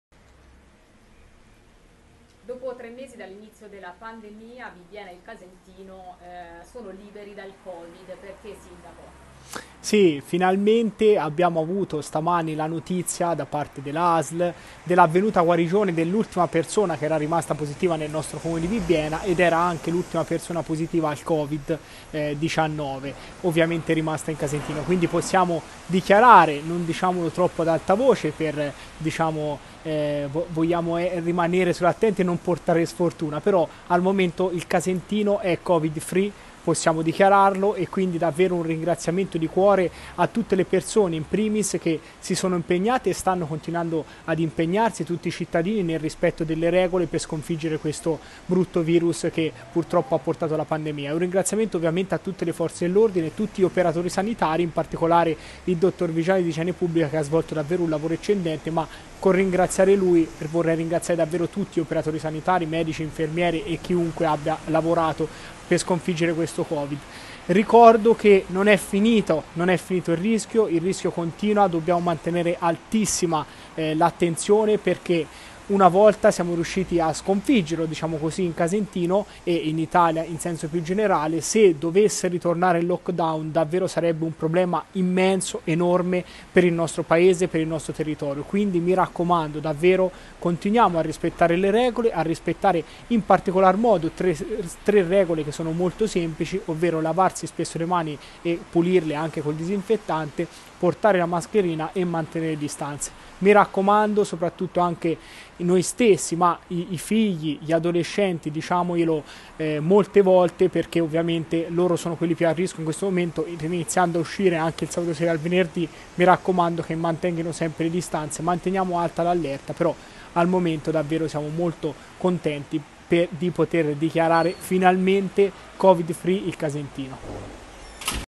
Il Casentino è Covid-free: dopo 3 mesi la vallata è libera dal virus
Filippo Vagnoli: Casentino Covid free